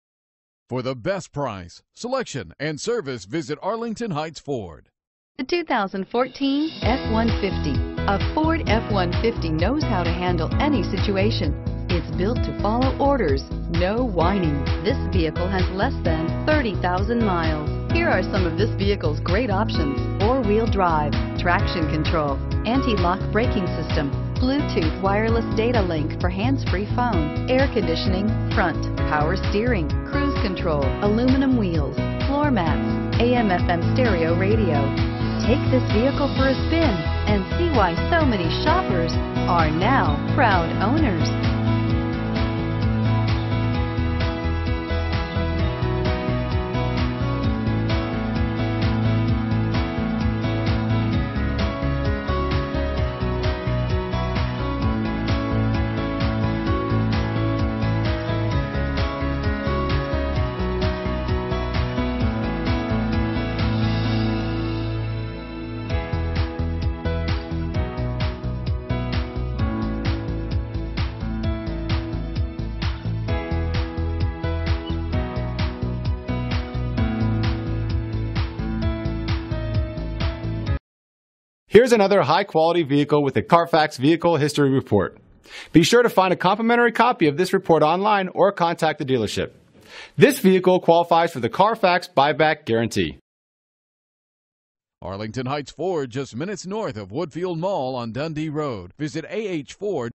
2014 Ford F-150 Live Arlington Heights IL 00P17949
2014 Ford F-150 XLT

801 Dundee Rd.

5.0L V8, Auto, 4WD, Alloy Wheels, Chrome Wheels, Rear Camerabr